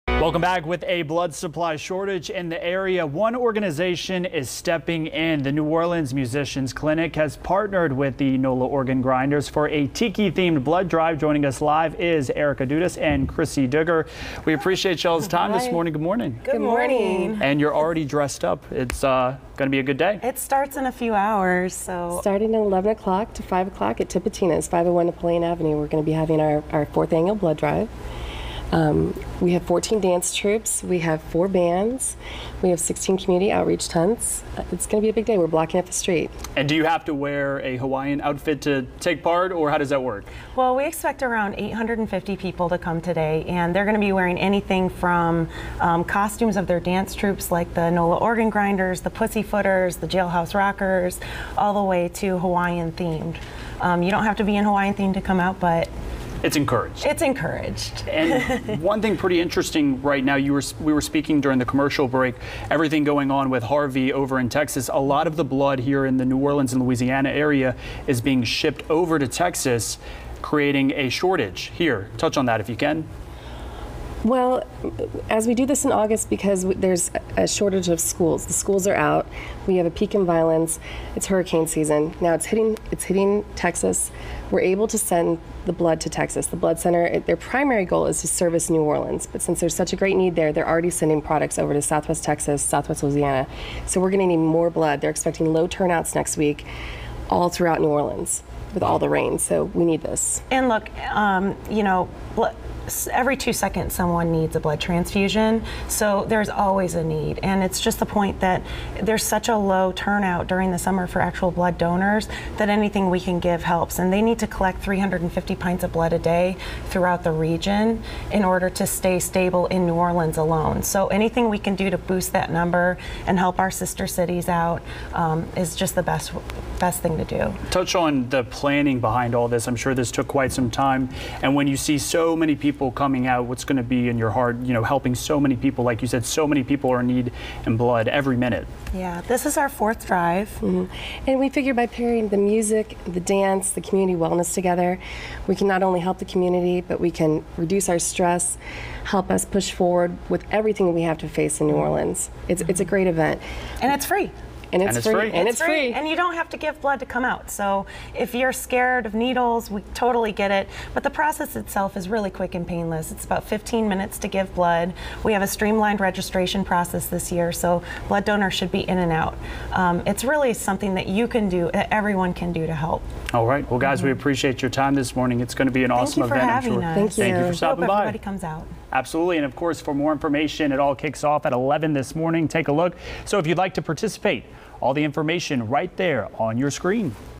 Dancers Unite for Summer Blood Drive
With a blood supply shortage in the area, one organization is stepping in. The New Orleans Musicians' Clinic has partnered with the NOLA Organ Grinders for their 4th annual summer blood drive.